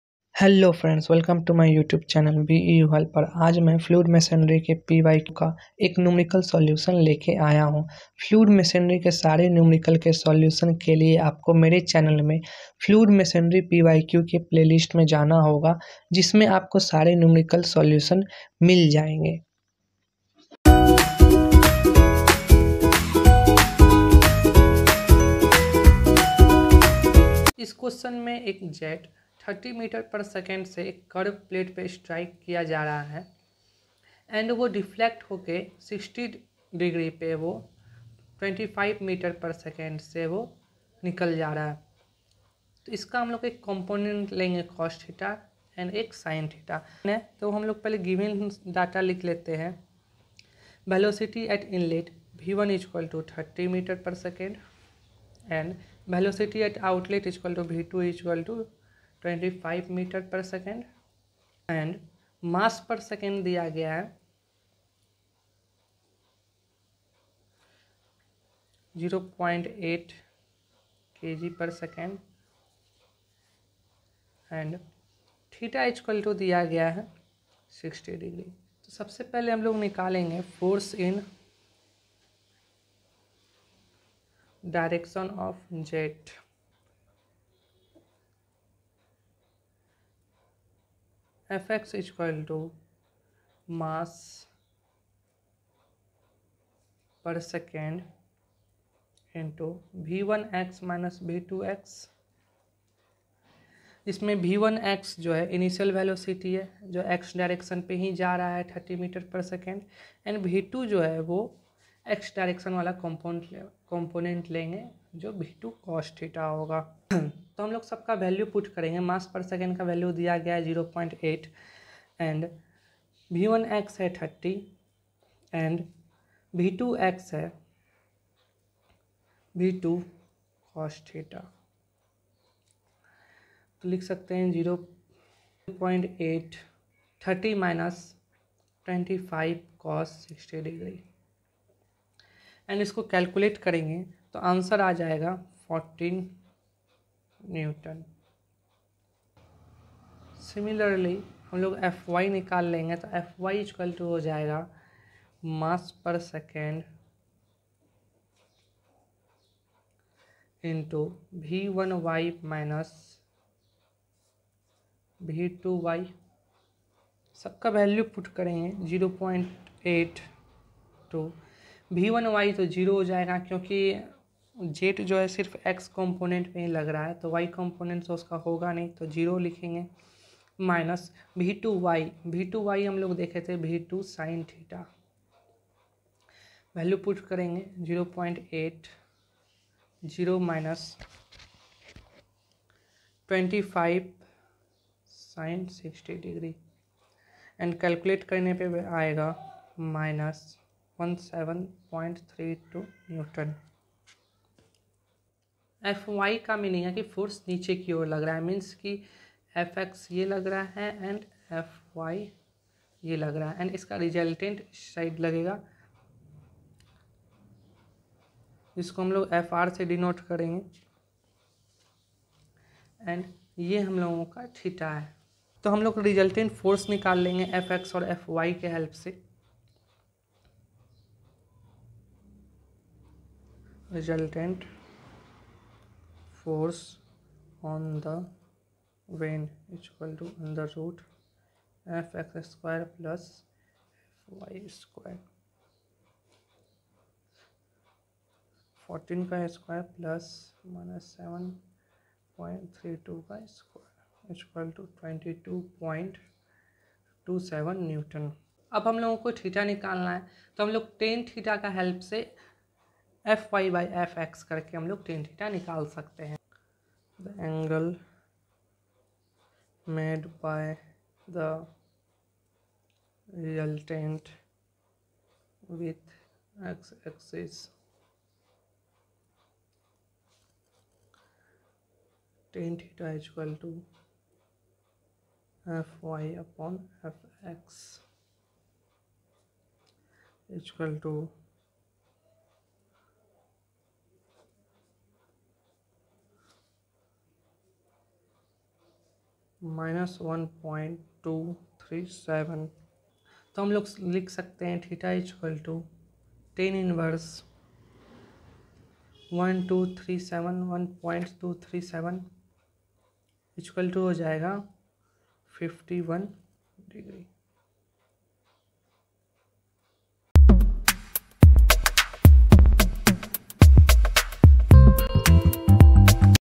Fluid machinery pyq 2021 || Numerical || BEU PYQ solution || FM || AKU
fluid machinery pyq 2021 solution
beu pyq 2021 solution
A jet of water from a nozzle is deflected through 60° from its original direction by curved plate which it enters tangentially without shock with a velocity of 30 m/s and leaves with a mean velocity of 25 m/s. If the discharge from the nozzle is 0.8 kg/s, calculate the magnitude and direction of the resultant force on the vane, if the vane is stationary.

This video is helpful for all btech student for all branches.
so please watch the complete video for your good marks in examination.